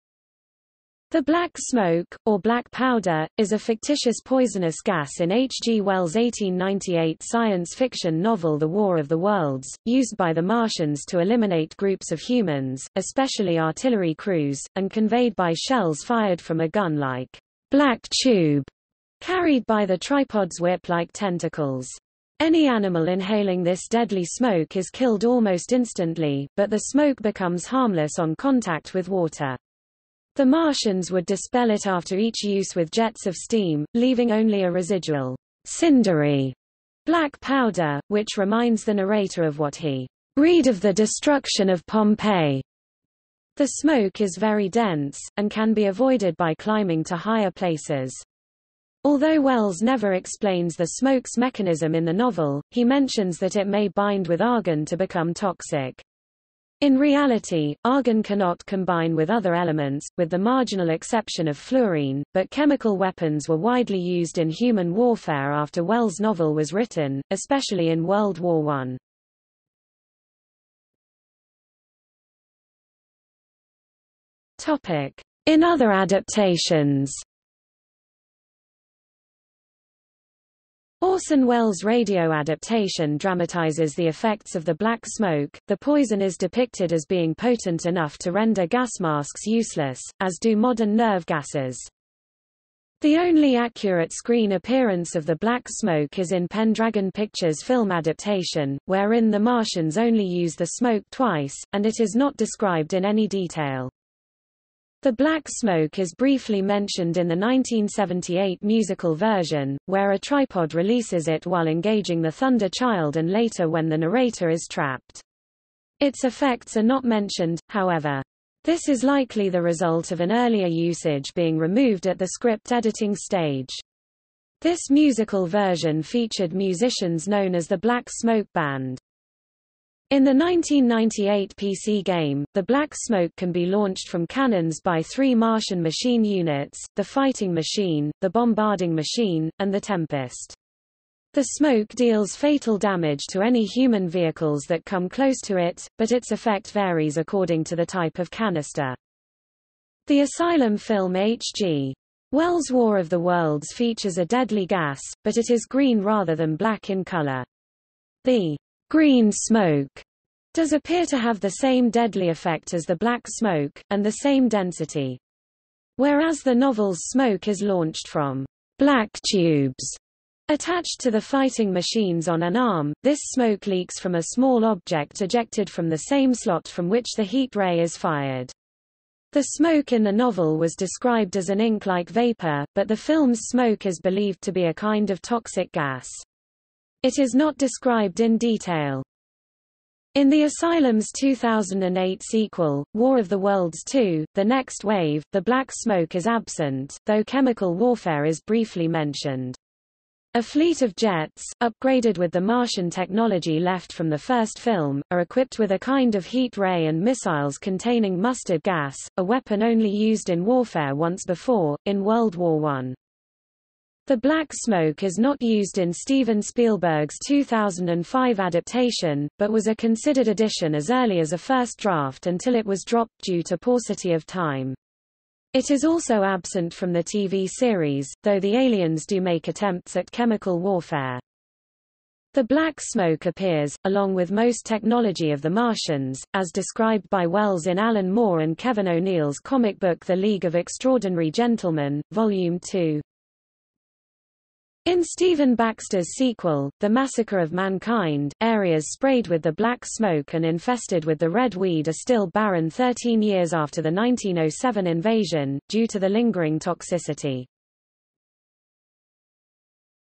Black smoke (The War of the Worlds) | Wikipedia audio article
00:01:10 In other adaptations

- Socrates

SUMMARY
The black smoke, or black powder, is a fictitious poisonous gas in H. G. Wells' 1898 science fiction novel The War of the Worlds, used by the Martians to eliminate groups of humans, especially artillery crews, and conveyed by shells fired from a gun-like "black tube" carried by the Tripods' whip-like tentacles. Any animal inhaling this deadly smoke is killed almost instantly, but the smoke becomes harmless on contact with water. The Martians would dispel it after each use with jets of steam, leaving only a residual, "cindery" black powder, which reminds the narrator of what he "read of the destruction of Pompeii". The smoke is very dense, and can be avoided by climbing to higher places. Although Wells never explains the smoke's mechanism in the novel, he mentions that it may bind with argon to become toxic. In reality, argon cannot combine with other elements, with the marginal exception of fluorine, but chemical weapons were widely used in human warfare after Wells' novel was written, especially in World War I.